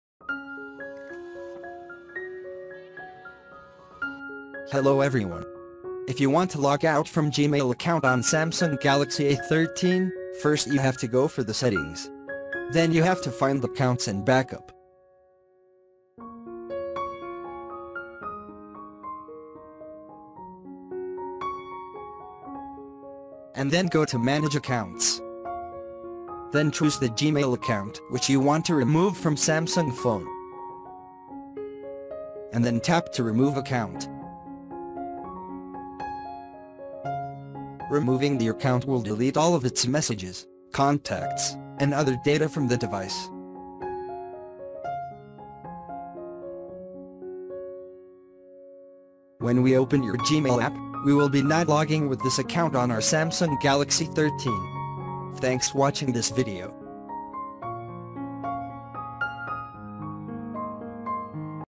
How to Remove Gmail Account on Samsung Galaxy A13 | Delete your Google Account from Samsung Phone
Learn how to pernamently delete Gmail Account on Samsung Galaxy A13. This video will show you how to remove Google Account from Samsung Galaxy A13.